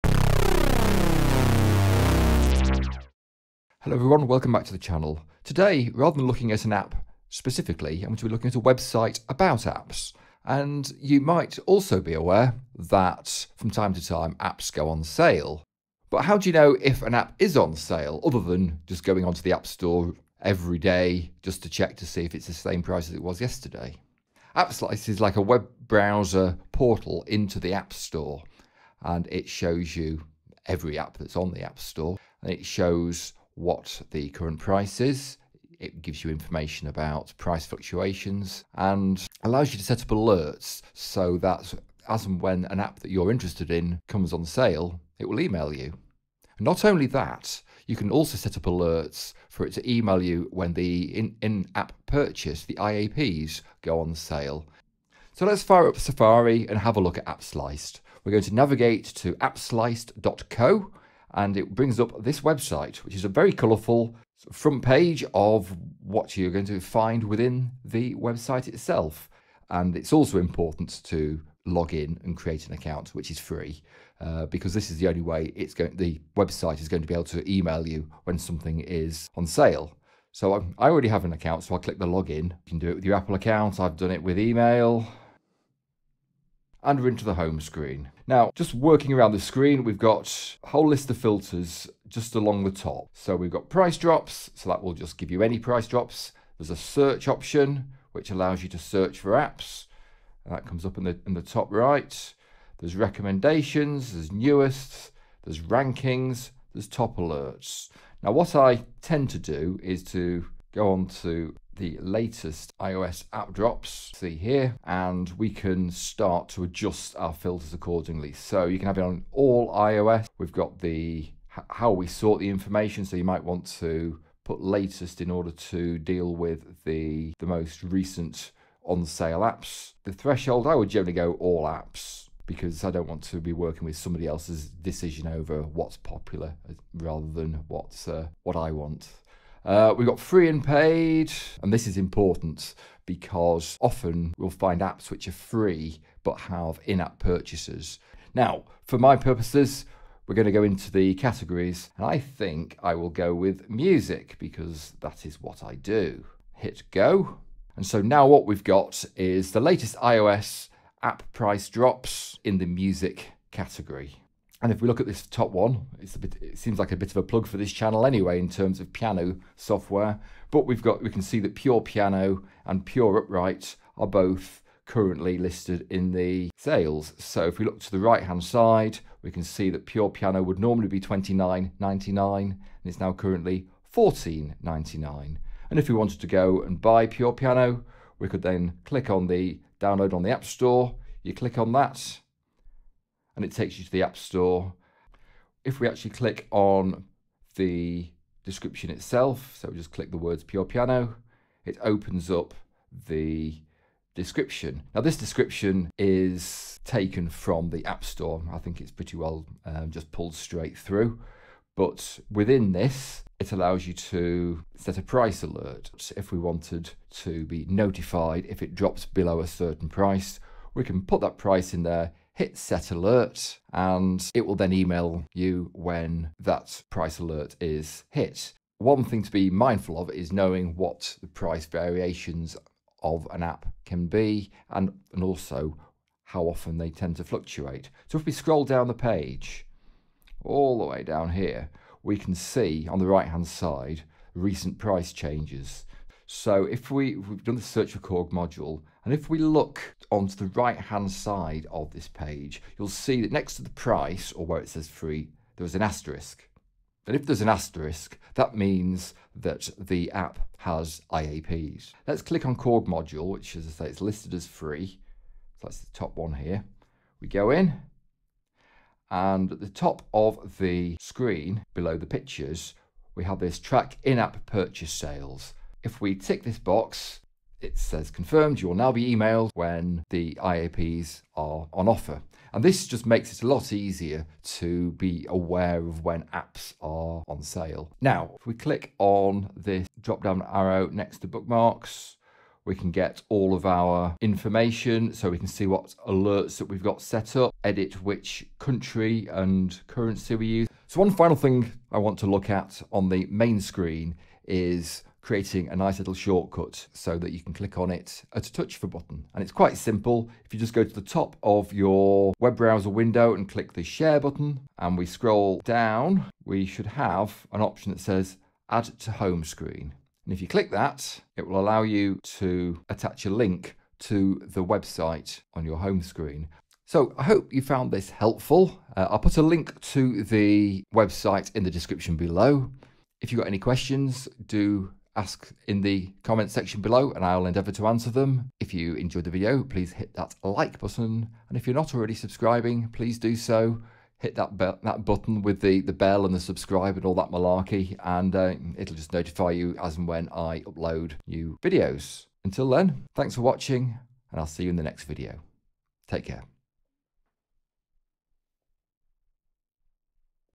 How to find cheaper apps on iOS
Appsliced was a freely available website which allows you to find and track prices of apps and in-app purchases (IAP). The site has since been retired in favour of their AppRaven app.

This is a quick tutorial to help you navigate around the site and get it setup for your own needs.

I'm a musician hence I keep an eye on the music apps. But this works for all apps on the App Store.